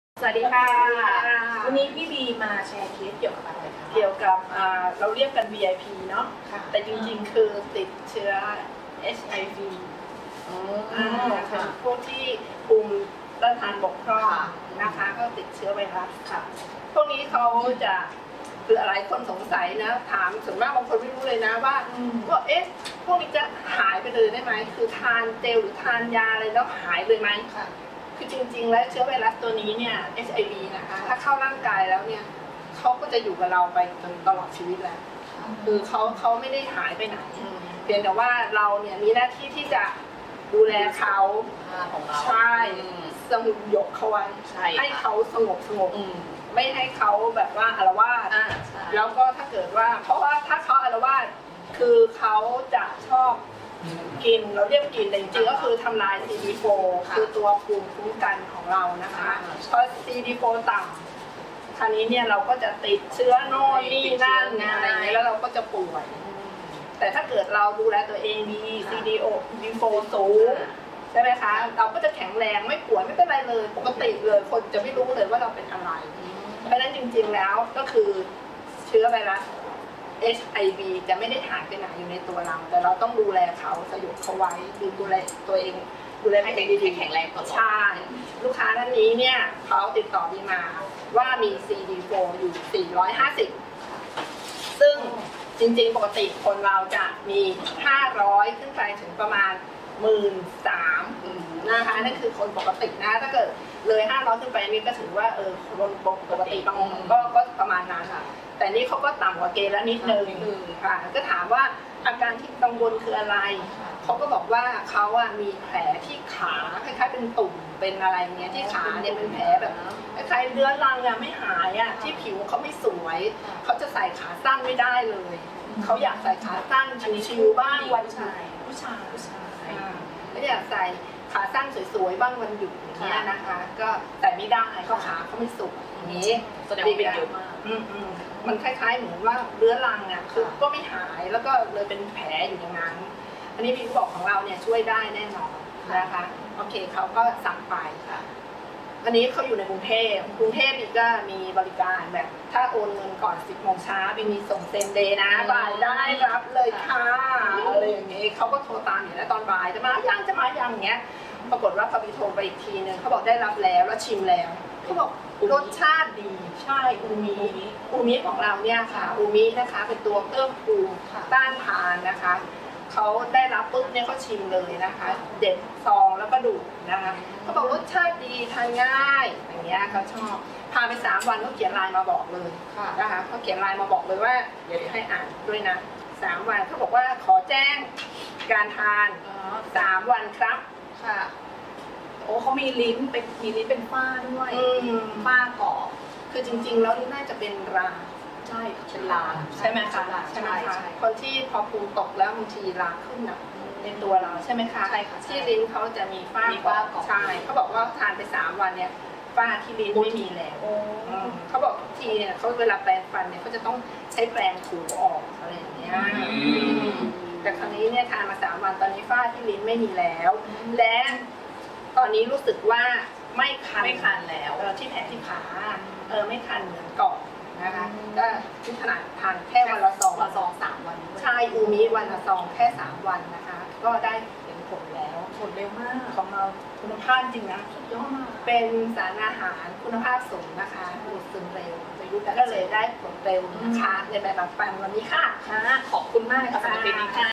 ติดเชื้อ HIV เอดส์ มีตุ่มแผล ลิ้นมีฝ้าขาว ทาน UMI ฝ้าขาวที่ลิ้นหาย ตุ่มคันหาย
สารอาหารแบบเจลดีกว่า ผง เม็ด น้ำ อย่างไร👇

เสริมภูมิ เพิ่มภูมิคุ้มกัน ฟูคอยแดน สารอาหารแบบเจล gelplus UMI